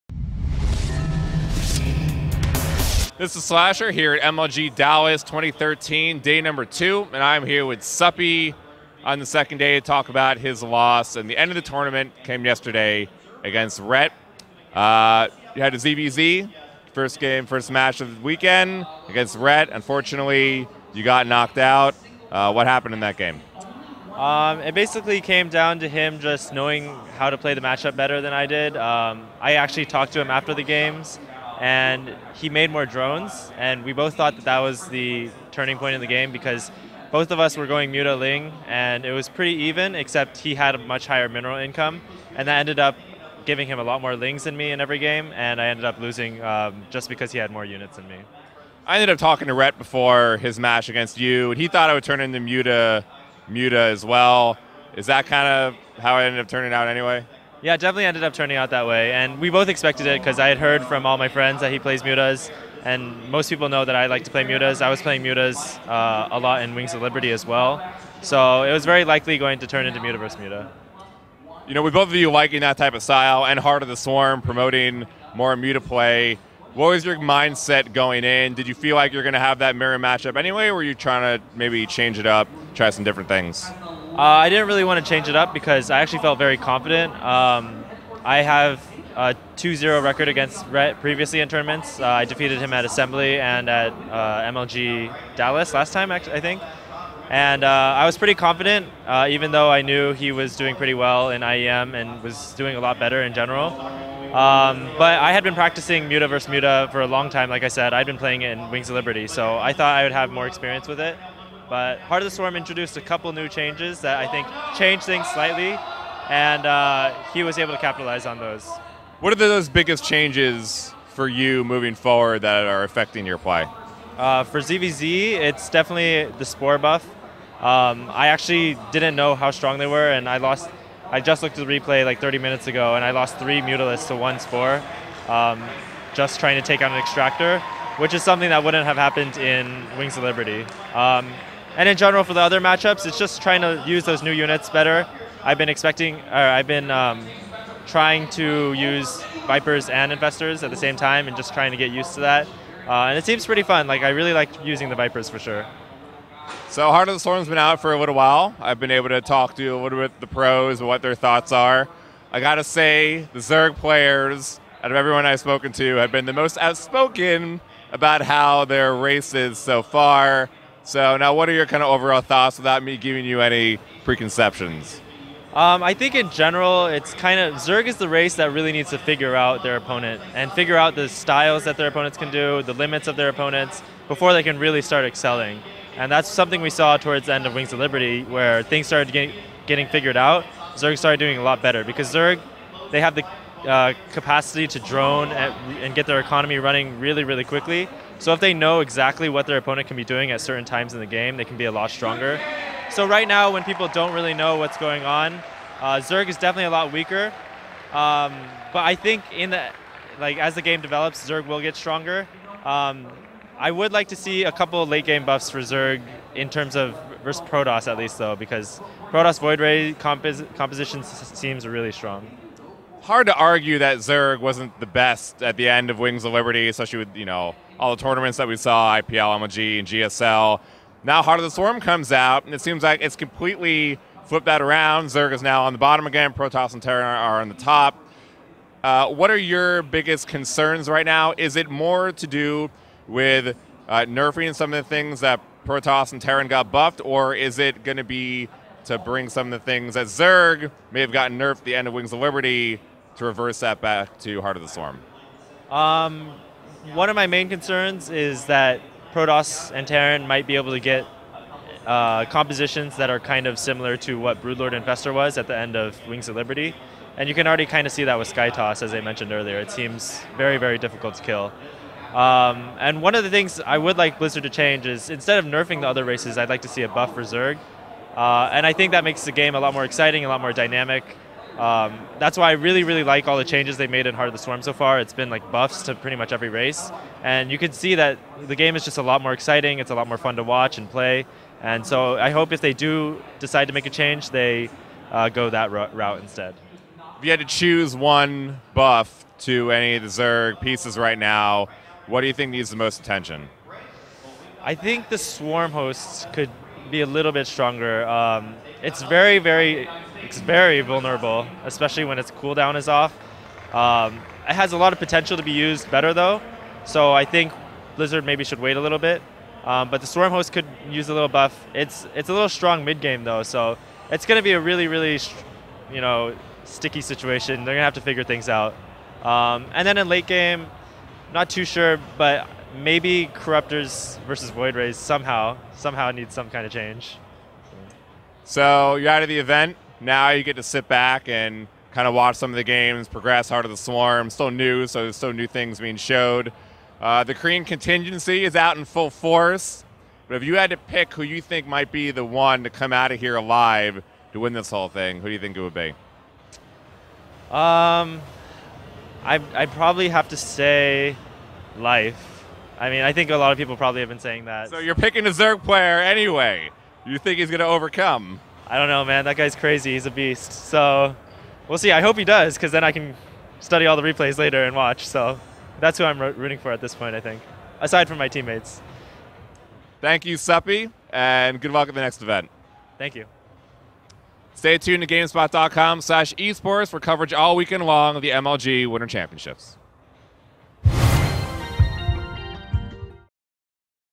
Suppy Would Like Zerg Buffs - MLG Dallas
Slasher asks Suppy about his tournament in Dallas and how HOTS is putting Zerg at a disadvantage.